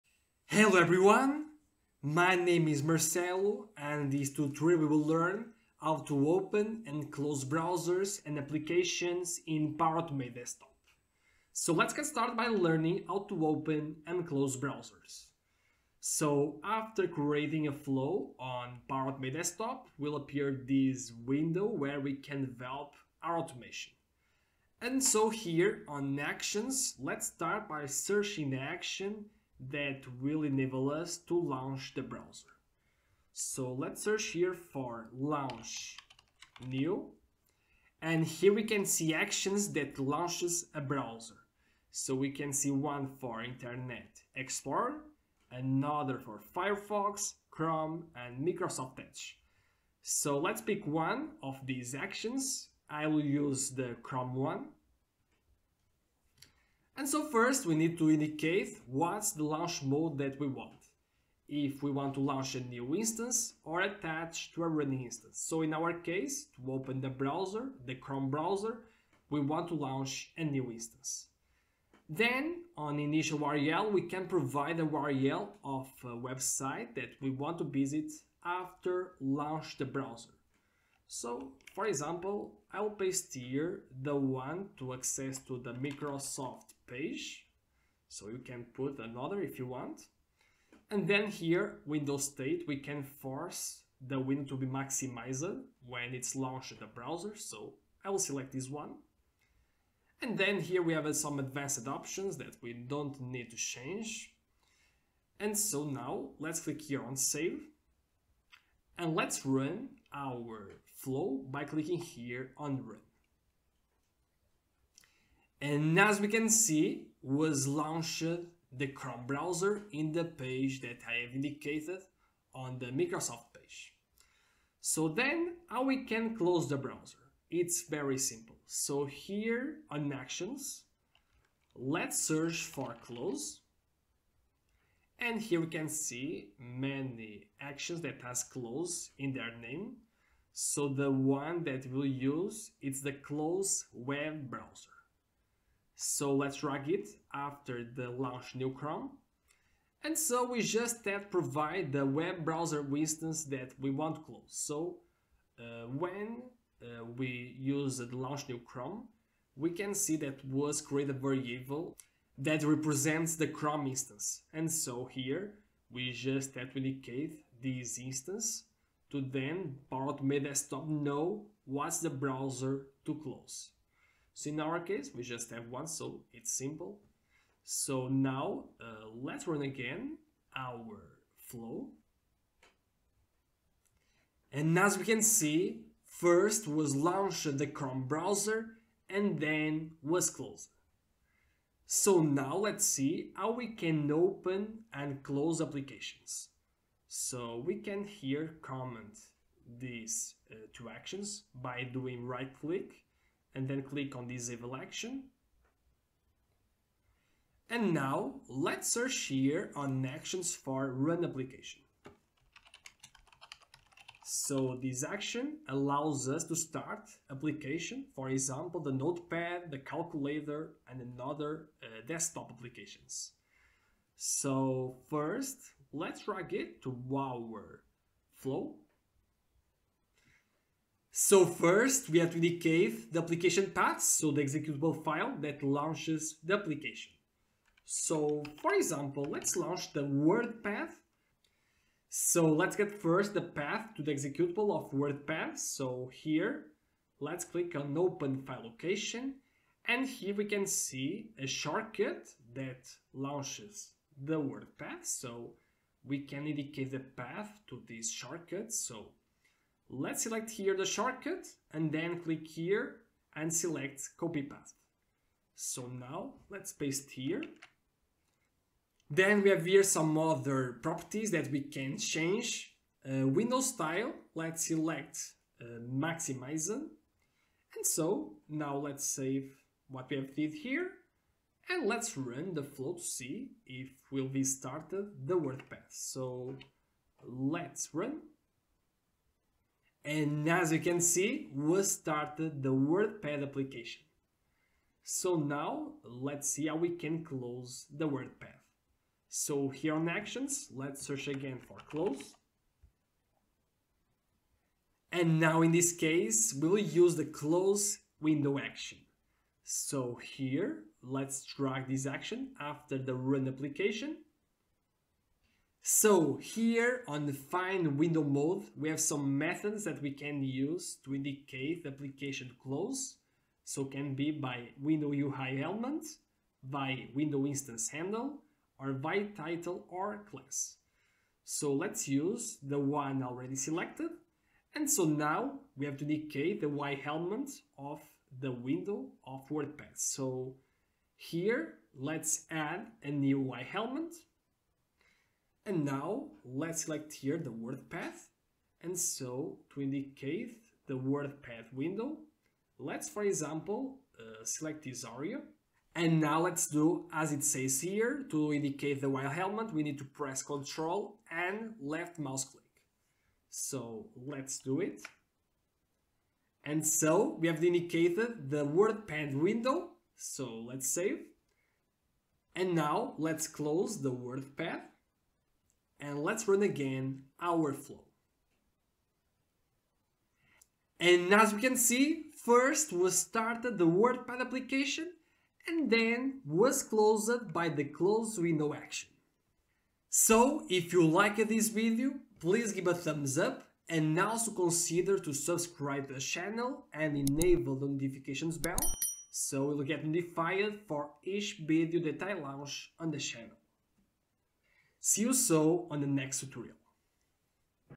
Power Automate Desktop - How To Open/Close Browser and Application
On this tutorial, we will learn how to open/close browser and application in the RPA tool Power Automate Desktop.

[00:10] -Introduction
[02:10] -Launch browser
[03:10] - Close browser
[04:54] - Open application
[06:30] - Close application
[06:47] - Cpnclusion